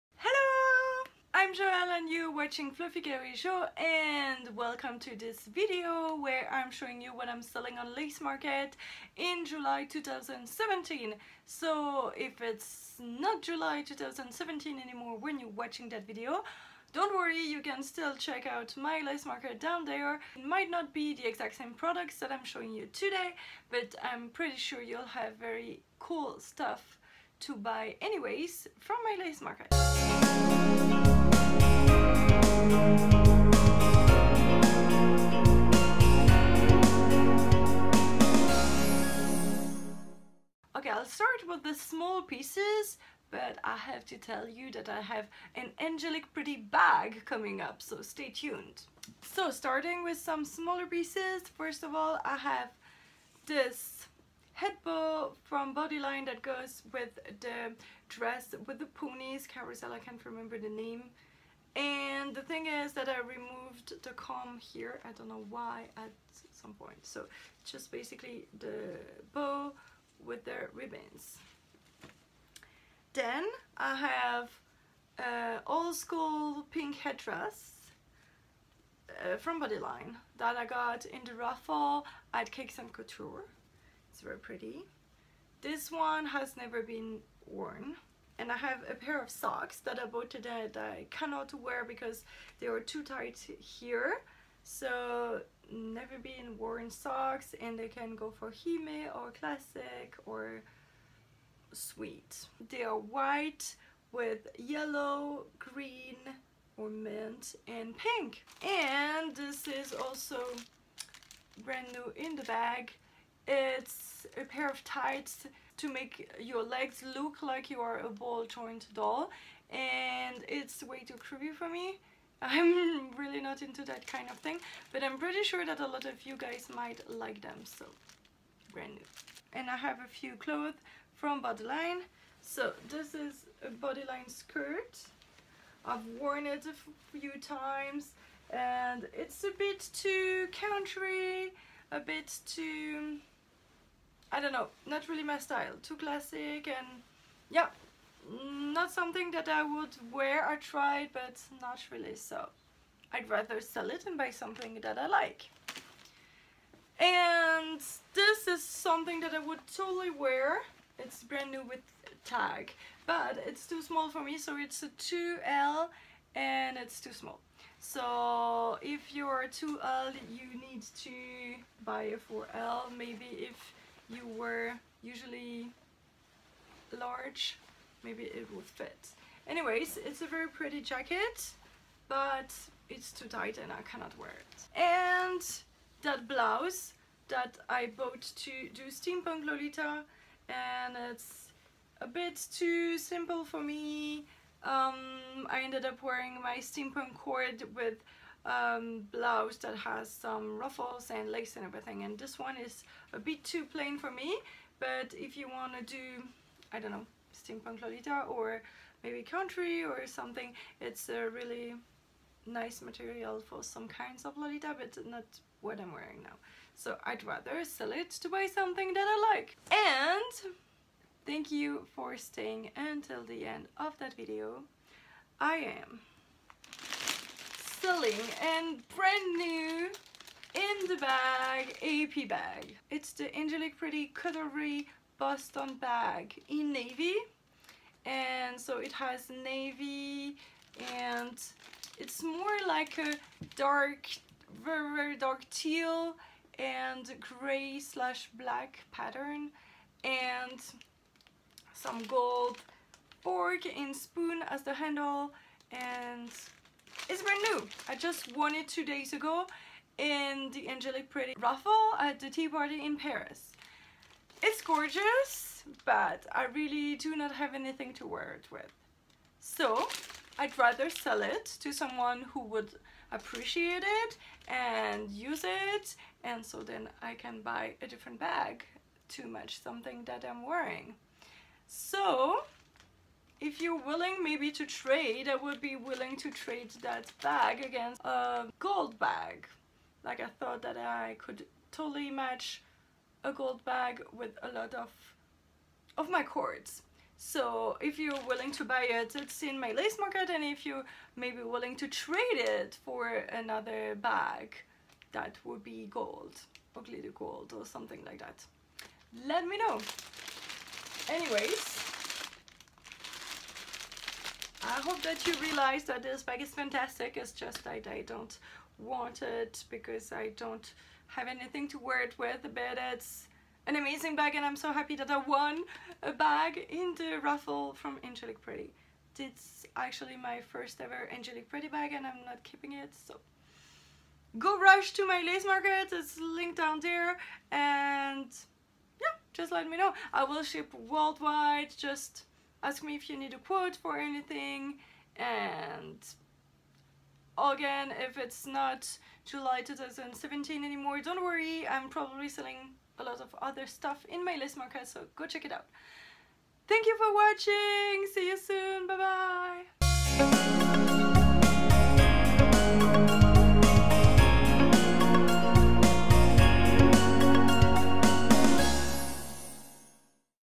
What I am selling on Lace Market - July 2017
Here's what I am selling on Lace Market in July 2017. Mostly Bodyline and Angelic Pretty.